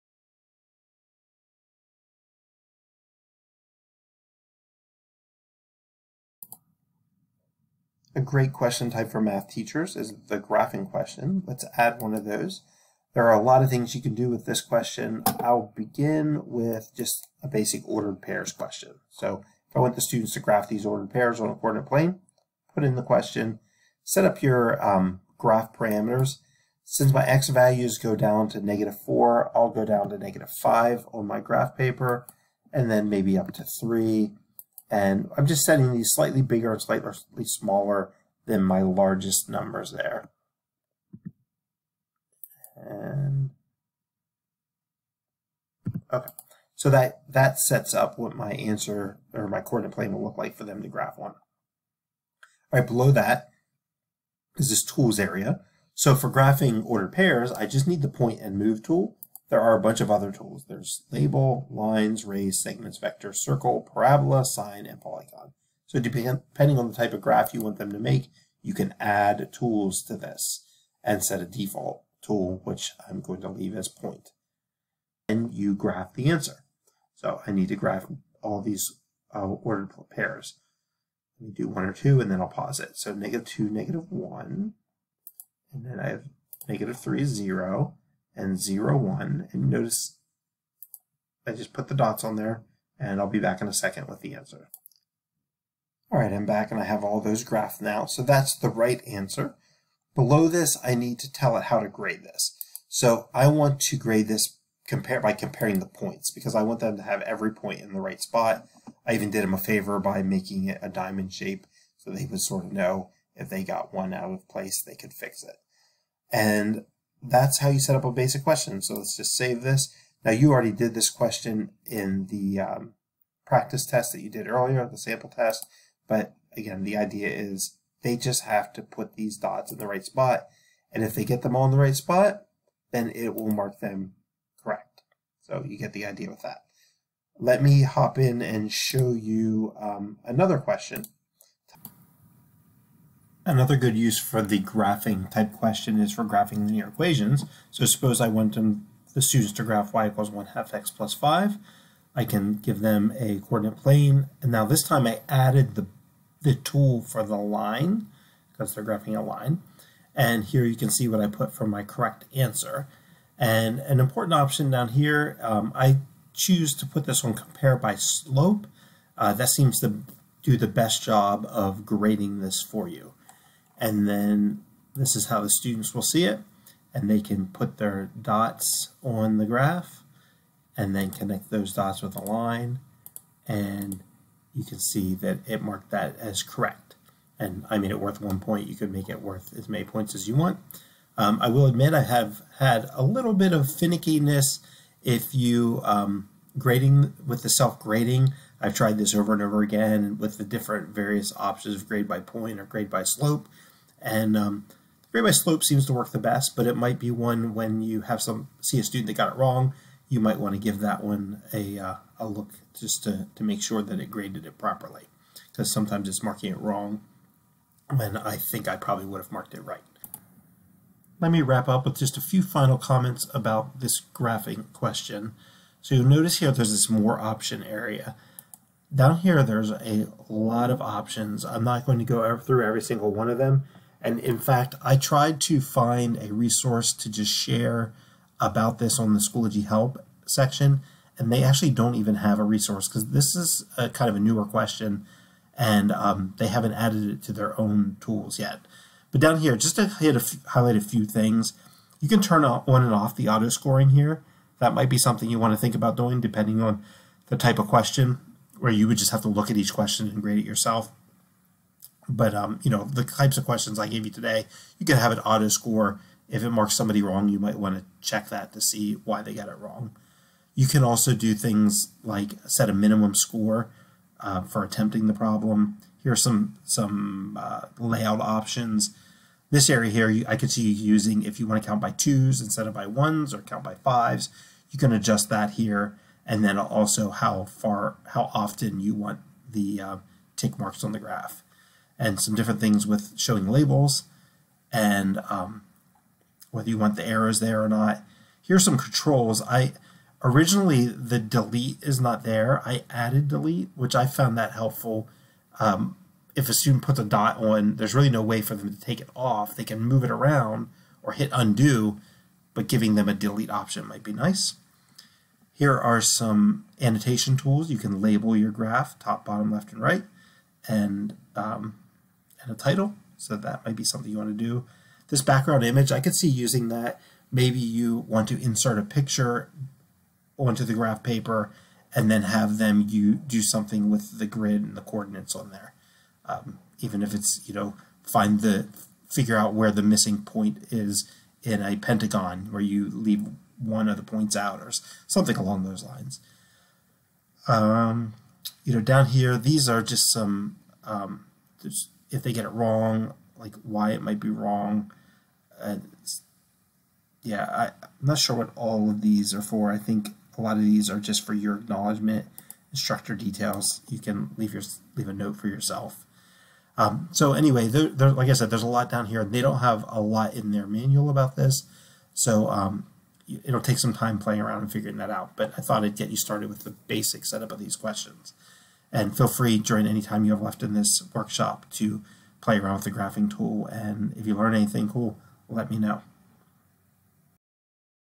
Schoology Assessments Graphing
This video will go over the basics of making a Schoology Assessment Graphing question